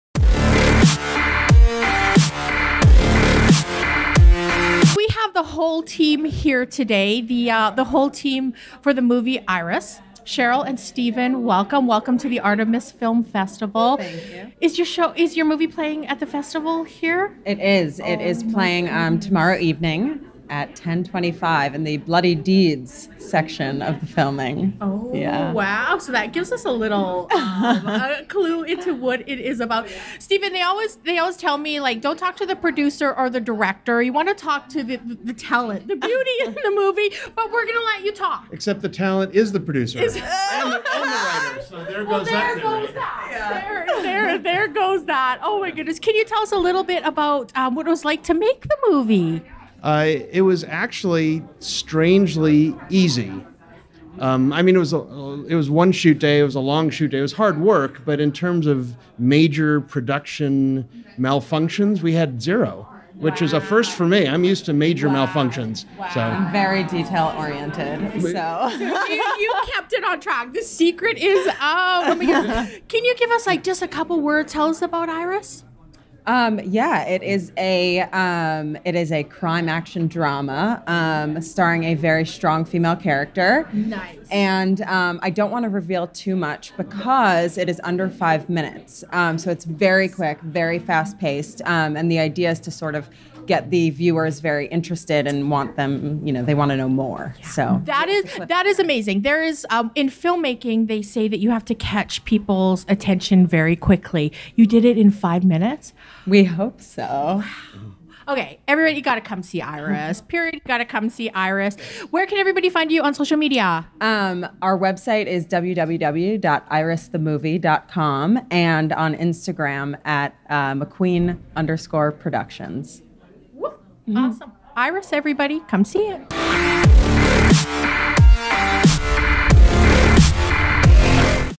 Sherry Lee Meredith interviews Cheryl McQueen Stephan David Brooks at the 2017 Artemis Red Carpet
Sherry Lee Meredith interviews actress/producer Cheryl McQueen and director Stephan David Brooks at the 2017 Artemis Red Carpet about their film Iris.